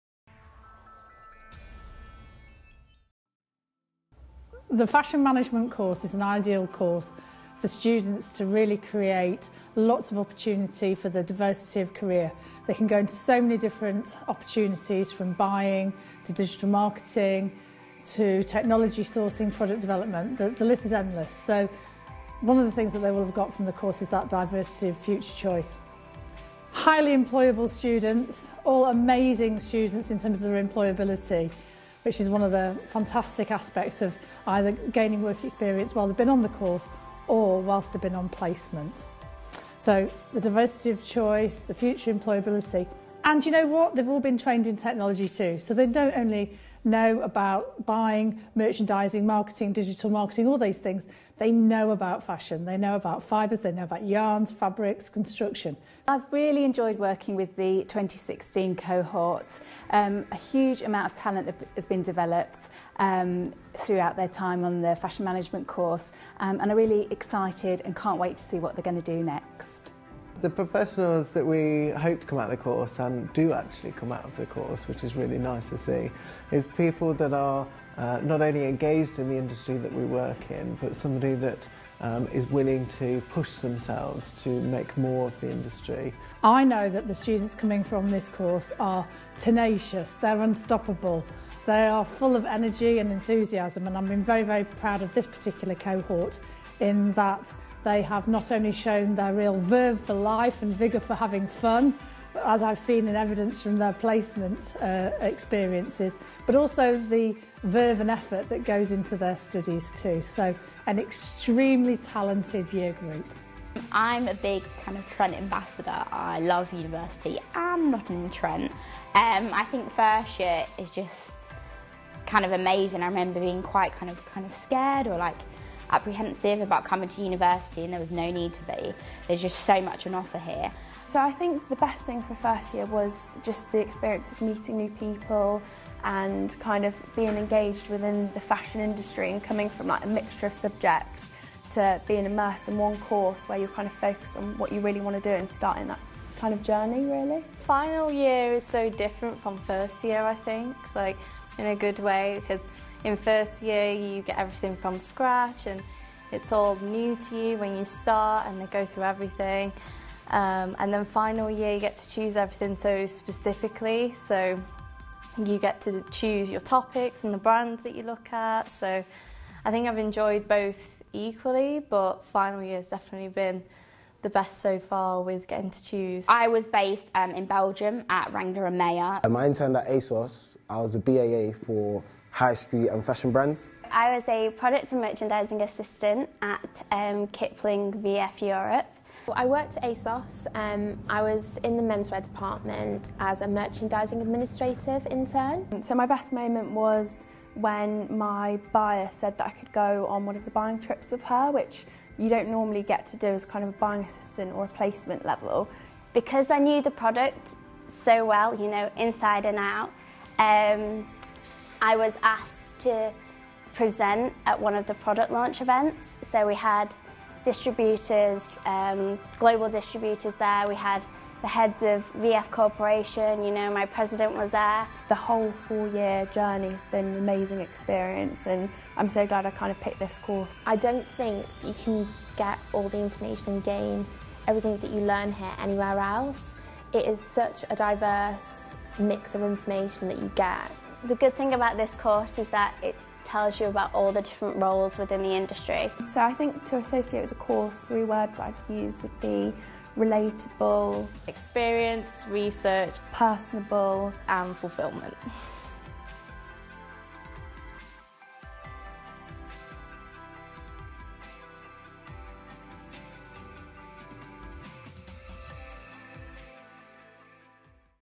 Nottingham Trent University Fashion Management 2016
As an exterior project, I was assigned to create a video for the Fashion Management course so it could be shown at their final year degree show.